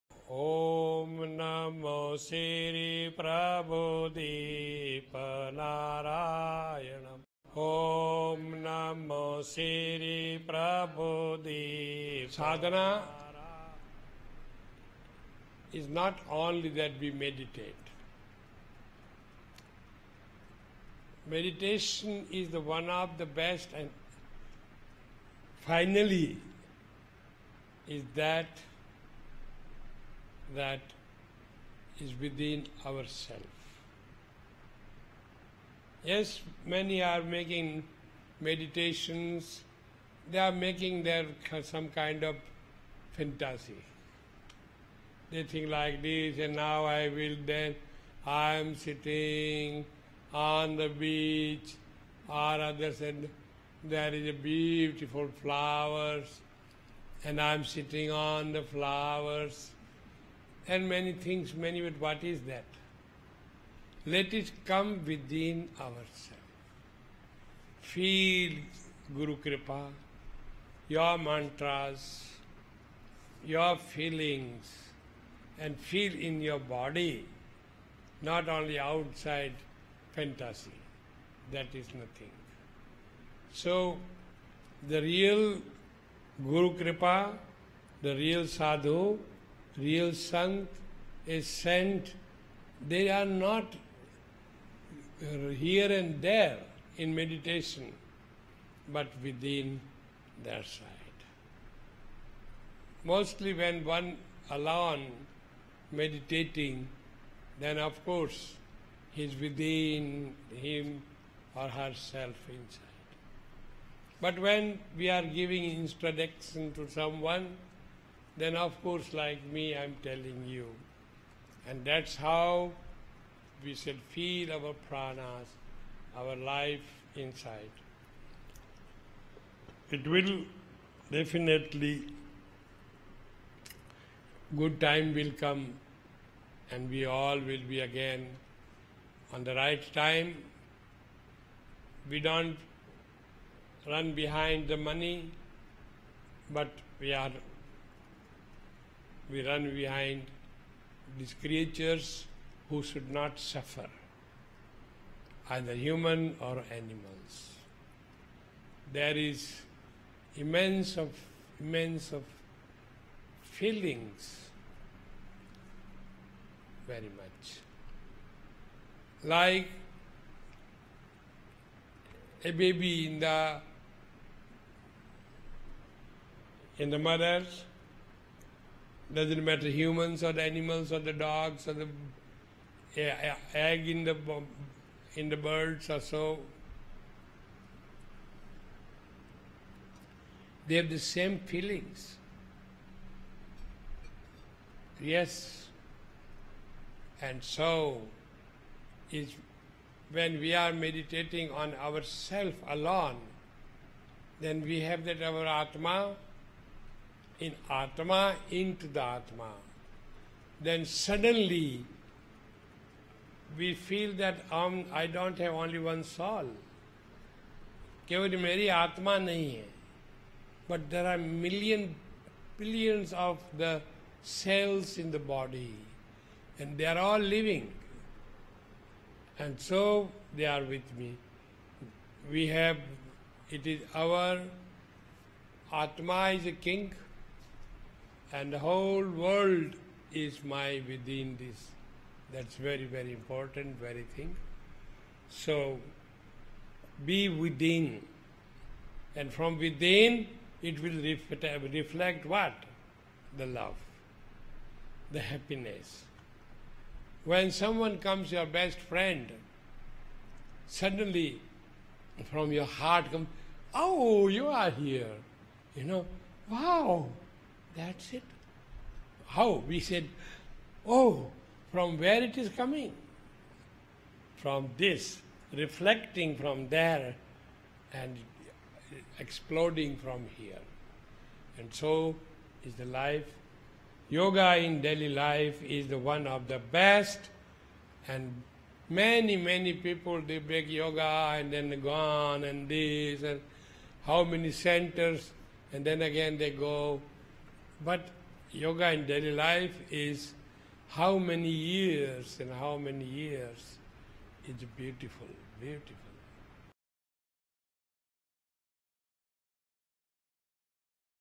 Inner Meditation will reflect Your Love
There is a big difference between meditating really inside or imagining beautiful images in the outer consciousness during meditation.

Vishwaguruji Paramhans Sri Swami Maheshwarananda,
spiritual master and yogi is the founder of Yoga in Daily Life System

Excerpt from Satsang in OM Ashram, Jadan, Rajasthan